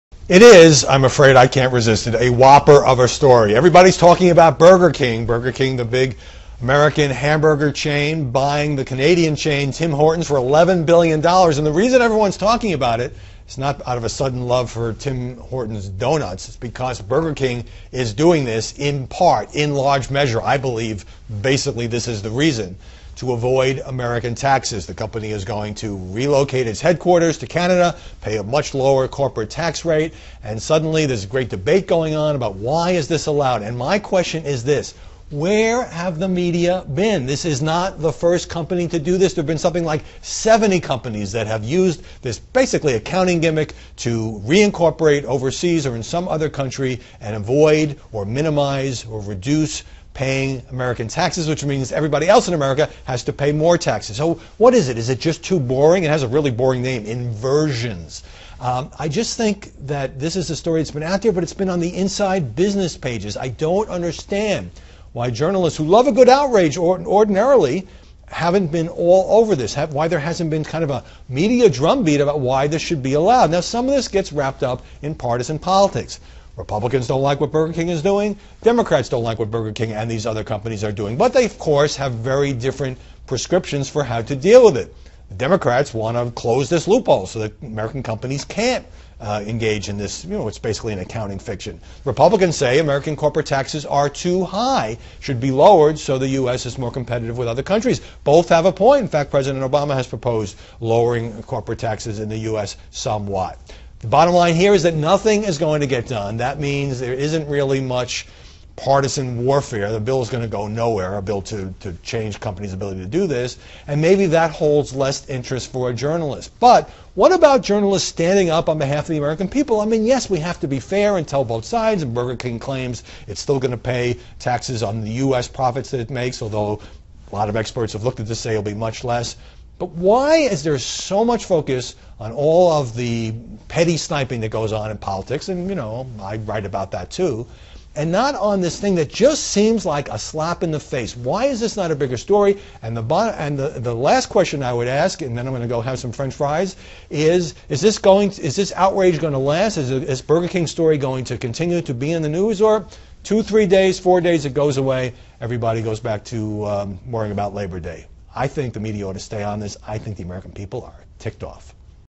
Kurtz on Burger King: Media wake up to corporate chicanry
It takes a whopper of a story for press to cover tax inversions.